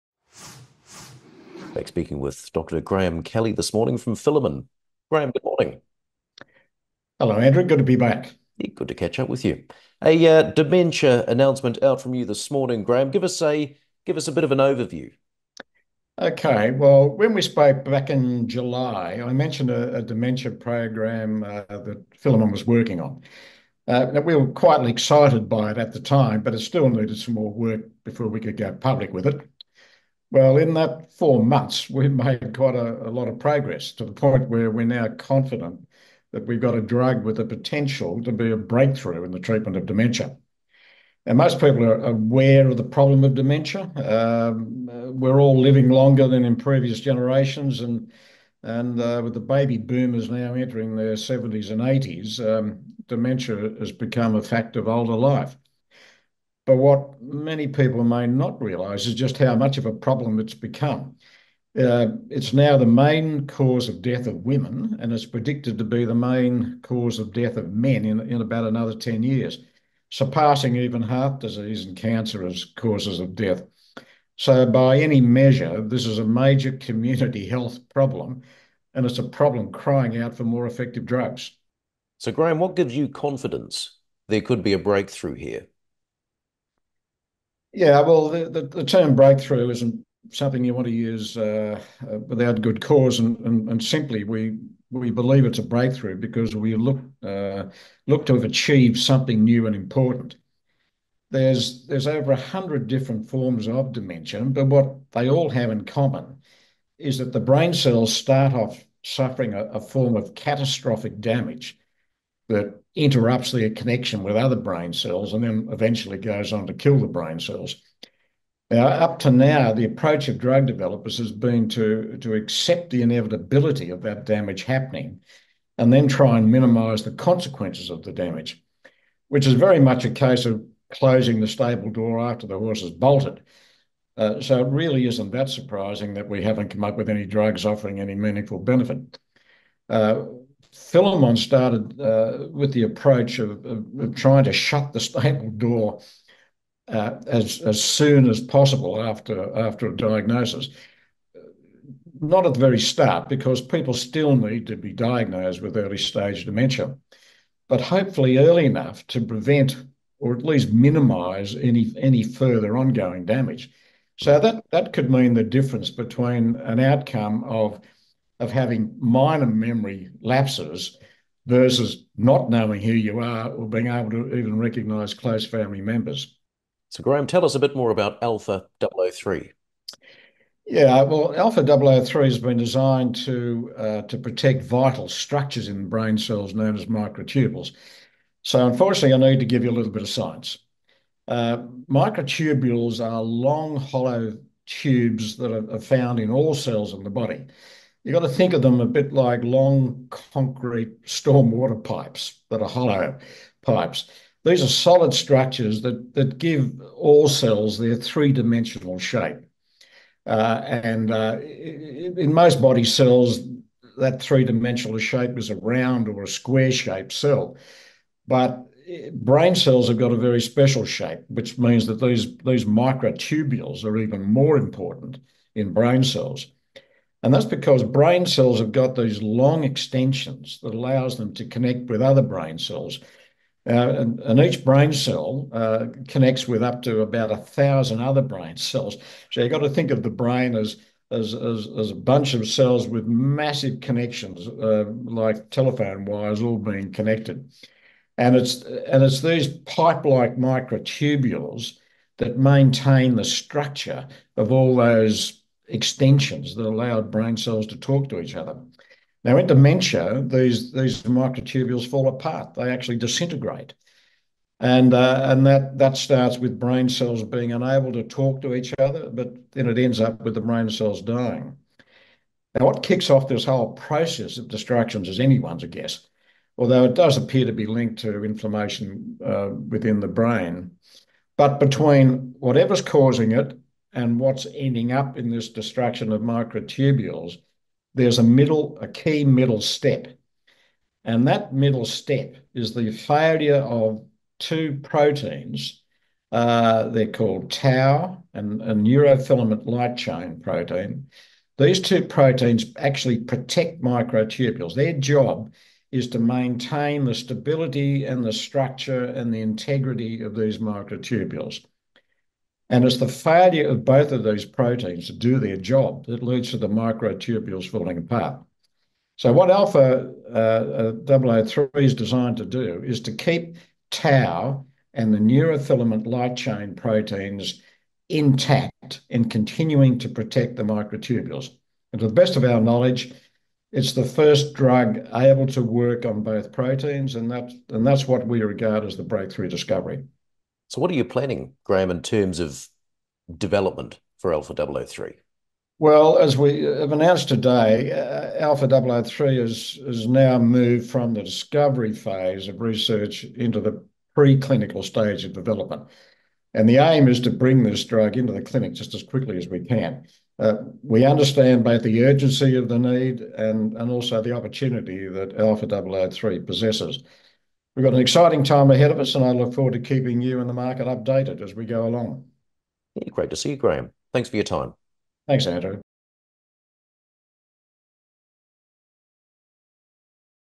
Filamon CEO Discusses With JustStocks Video Potential Breakthrough In Dementia Treatment
Filamon Ltd CEO Dr Graham Kelly speaks with JustStocks Video about what the company believes to be a major step forward in the search for a potentially effective treatment for dementia.

Video Highlights

Current approaches to dementia treatment that focus on minimising damage after brain cells have already suffered catastrophic structural damage treatment have failed to deliver meaningful benefits
Filamon sought to design a drug capable of being used early in the disease process to prevent the damage from getting any worse
ALPHA-003 is intended to preserve the integrity of brain cell microtubules by protecting them from destructive neuroinflammation
In a world-first achievement, ALPHA-003 is designed to protect both tau and neurofilament proteins from responding to inflammation that leads to microtubule degradation.

About ALPHA-003

ALPHA-003 is the result of Australian-designed, deep-learning, computational drug design technology.

In what is believed to be a world-first achievement, ALPHA-003 binds to both tau and neurofilament light chain with the aim of stopping them from being disrupted by destructive inflammatory forces.

Importantly, Filamon also has confirmed that ALPHA-003 is able to cross the mammalian blood-brain barrier.

ALPHA-003 is being developed to treat a group of diseases known as tauopathies which include the two major forms of dementia (Alzheimer’s disease, frontotemporal dementia), progressive supranuclear palsy (a form of Parkinson’s) and chronic traumatic encephalopathy (CTE, the result of repeated concussion).

To date, no approved treatment has emerged as offering any meaningful ability to slow down the rate of deterioration of tauopathy diseases.

Filamon is committed to bringing this exciting opportunity into the clinic as quickly as possible, with a range of clinic-enabling pre-clinical studies currently underway.